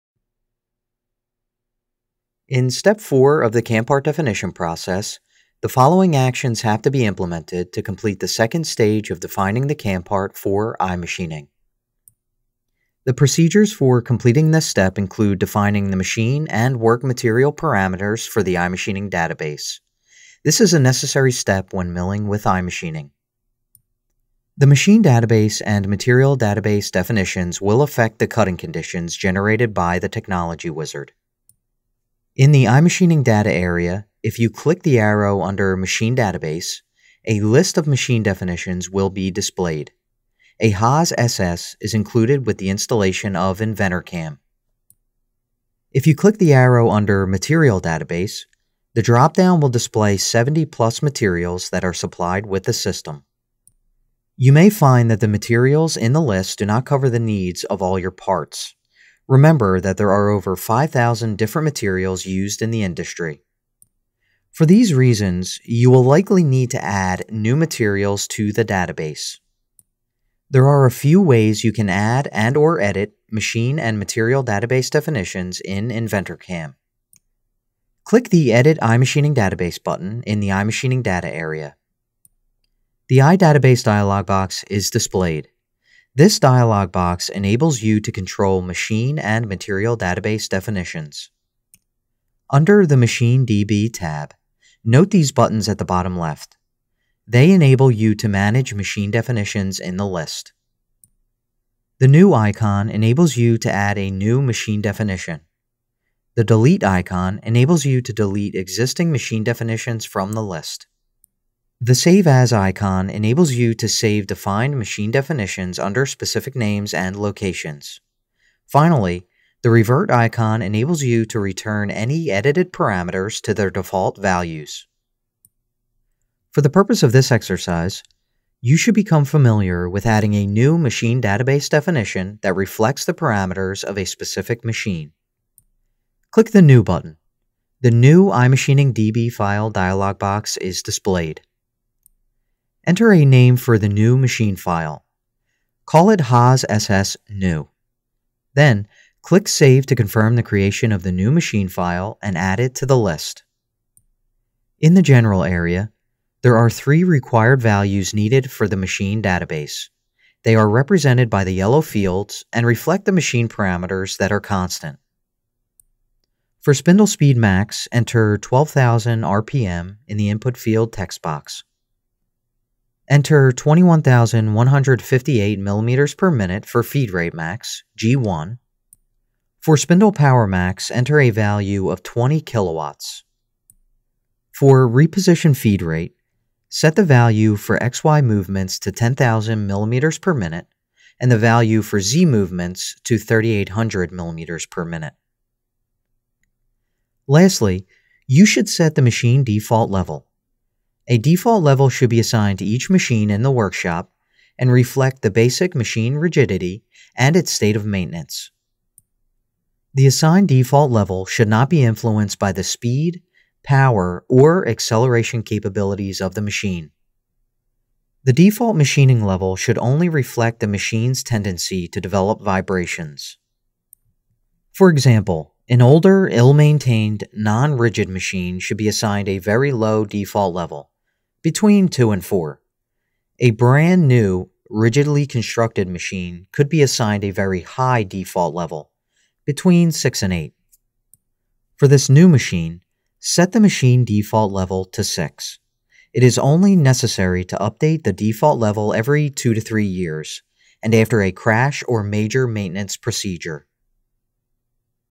InventorCAM iMachining Training Course - Exer #1: Machine Database definition
This video is Step 4.1 in Exercise #1 of the Interactive Training Course for iMachining. The following actions are implemented to complete the second stage of defining the CAM-Part (for iMachining). An overview of the iMachining Data is covered in the Milling Part Data dialog box. Completing the first part of Step 4 includes adding a new Machine Database file (Haas_SS_New), defining the machine parameters and setting the Machine Default Level (6).